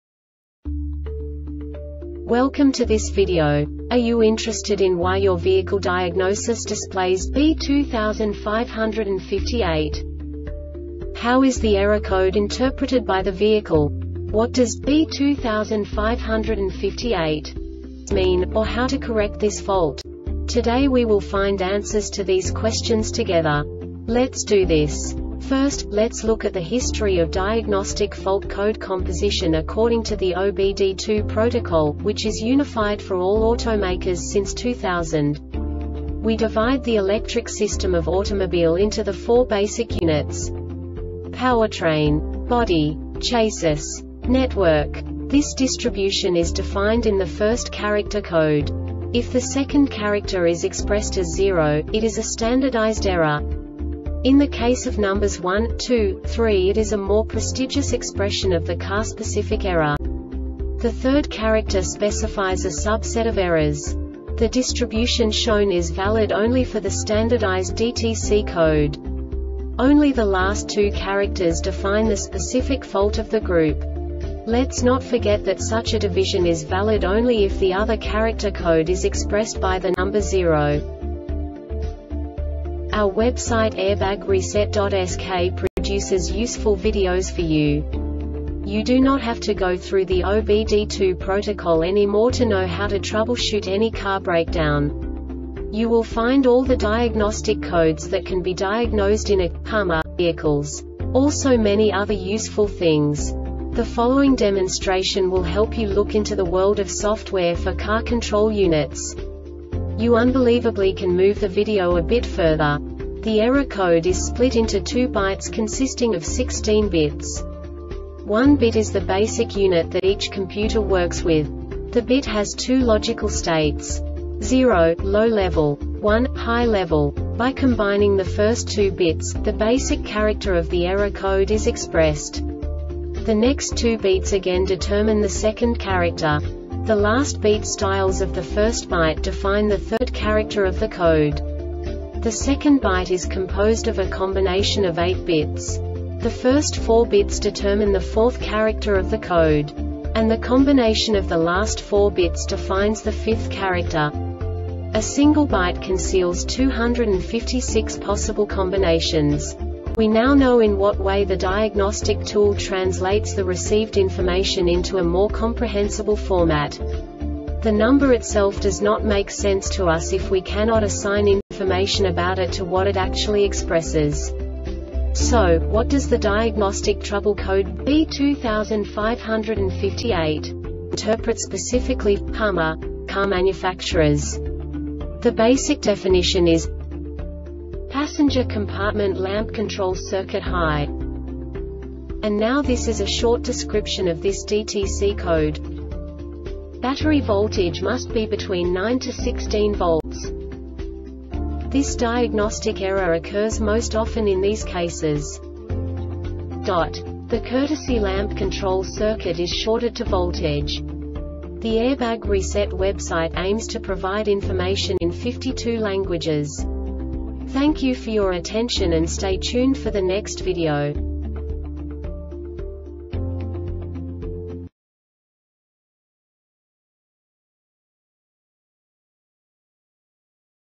DTC Hummer B2558 Short Explanation
Contents:
0:21 Basic DTC analysis according to OBD2 protocol standard.
1:48 Insight into programming
2:58 A diagnostically understandable description of the Diagnostic Trouble Code
3:05 Basic error definition

Make:
HUMMER
Code:
B2558
Definition:
Passenger Compartment Lamp Control Circuit High
Description:
Battery voltage must be between 9-16 volts.
Cause:
The courtesy lamp control circuit is shorted to voltage.